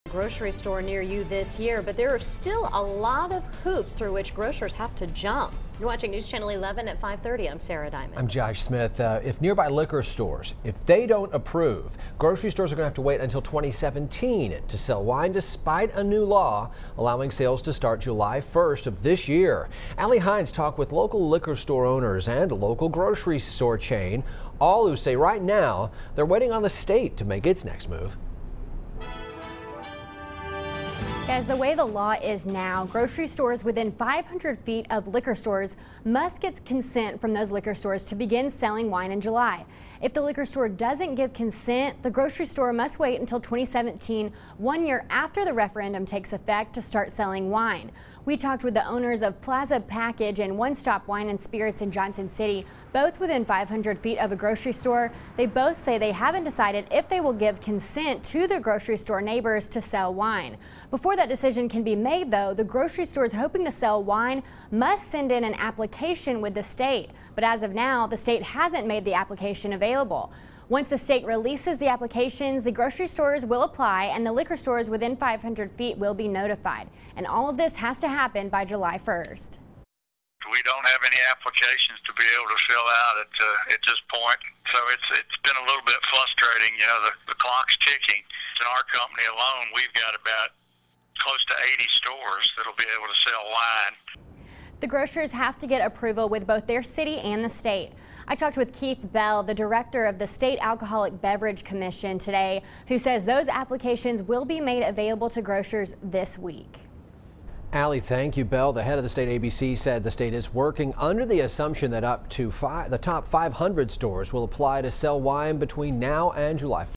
TN grocery stores waiting to start application to sell wine in July
Wine may be coming to a grocery store near you this year, but there are still hoops for grocers to jump through.